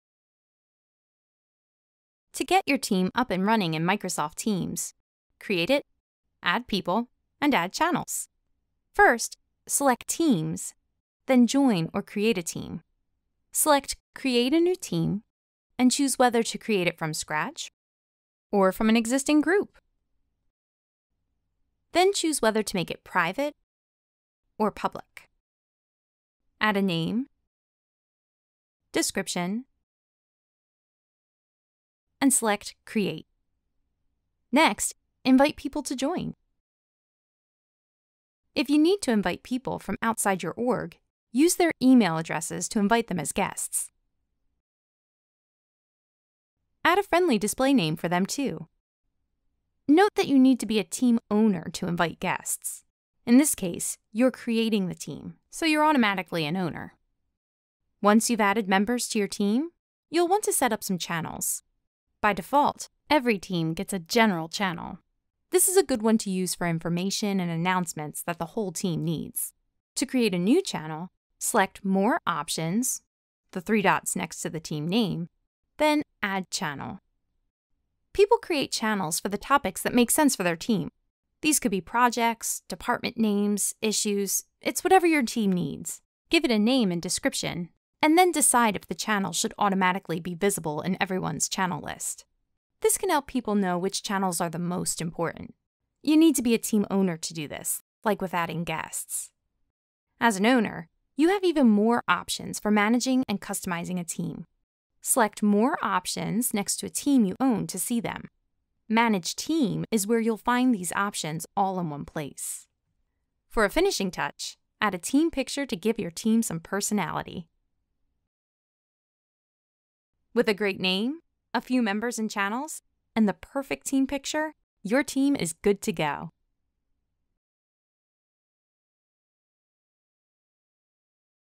How to get your team up and running in Microsoft Teams
As you’re working from home and managing remote employees, Microsoft Teams is a central hub for teamwork. This tutorial will how you how to set up and use Microsoft Teams to make the most of work from home productivity.

To get your team up and running in Microsoft Teams, create a team, add people, and add channels. After watching, you’ll walk away with all the steps to add members to your teams and channels, and even customize your settings to add a team picture and give your team some personality.